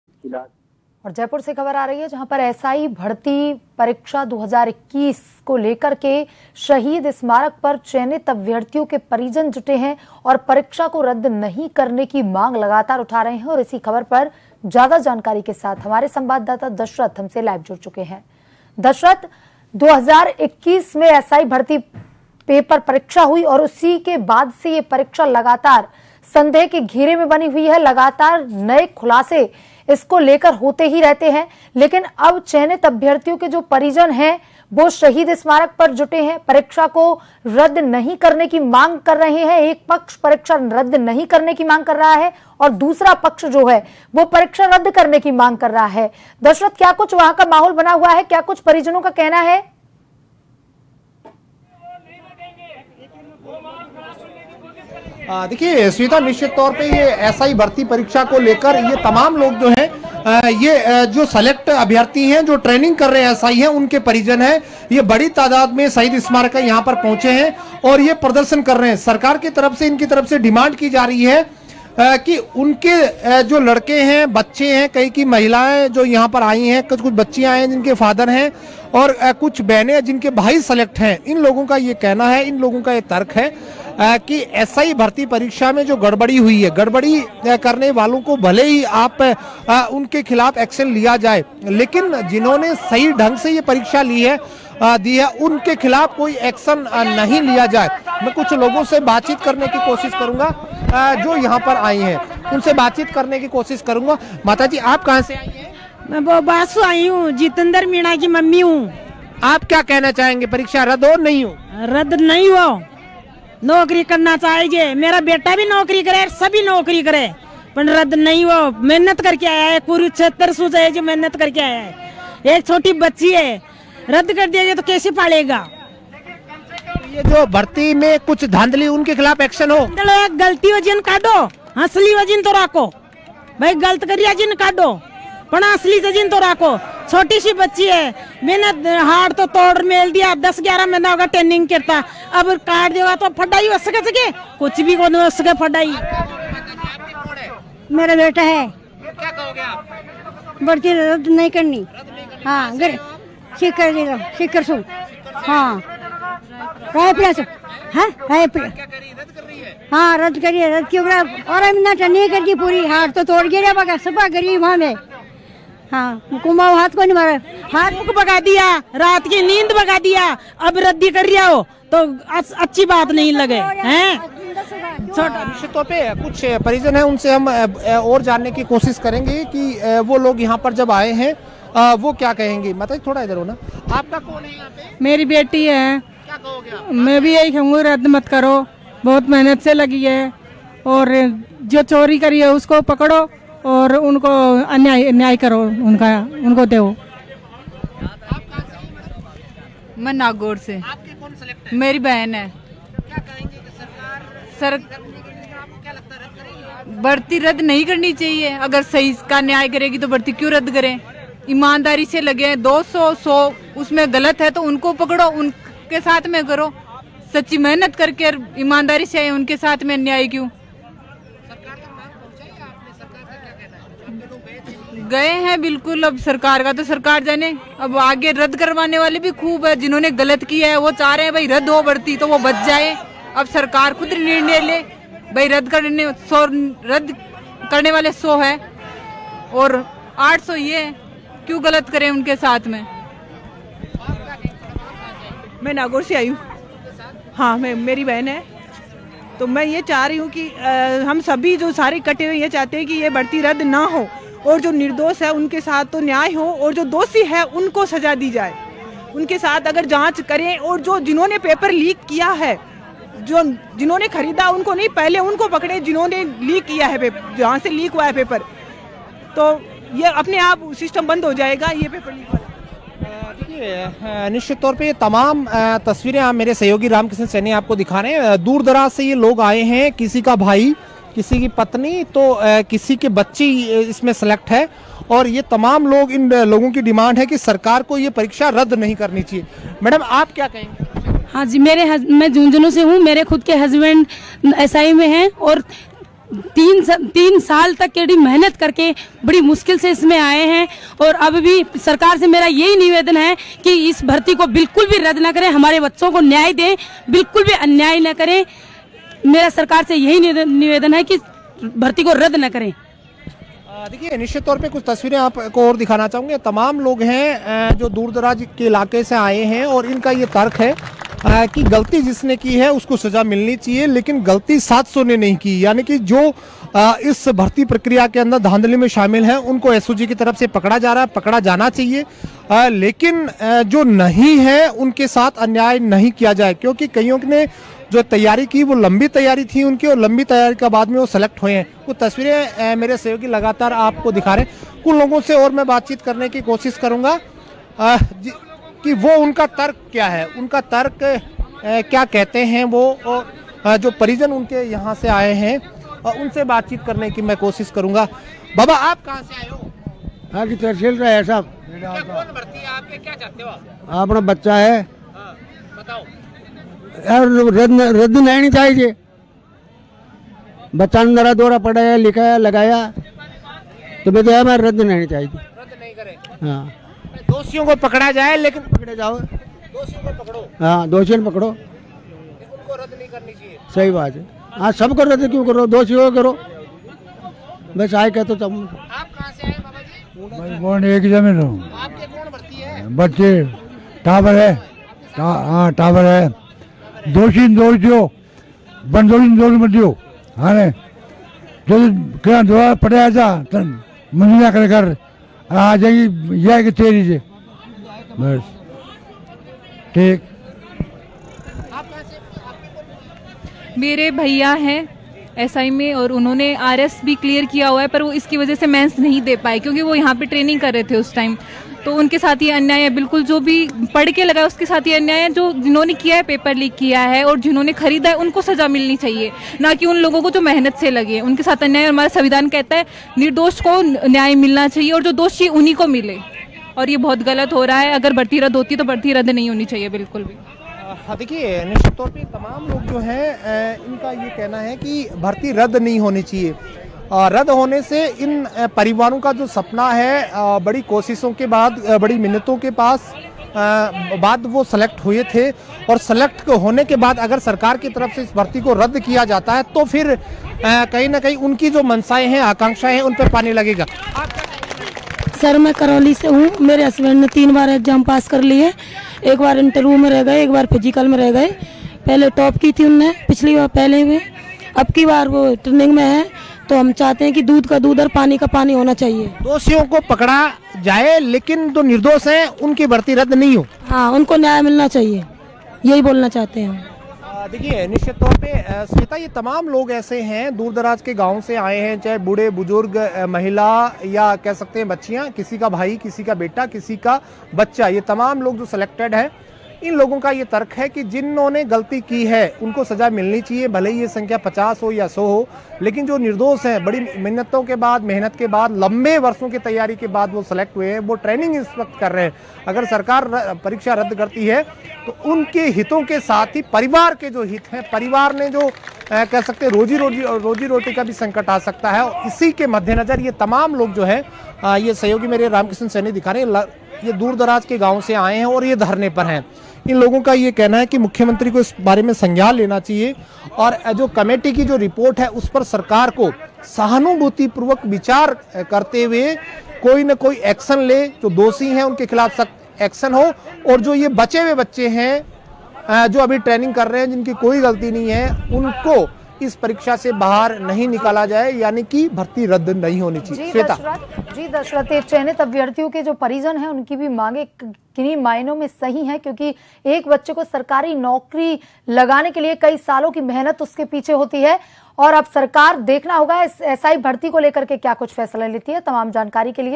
Jaipur Live | एसआई भर्ती 2021 मामला, शहीद स्मारक पर जुटे चयनित अभ्यर्थियों के परिजन | JAN TV
Watch JAN TV on :
Tata Play DTH : 1185
Airtel DTH: 355
JIO Fiber: 1384

News Credit-VKJ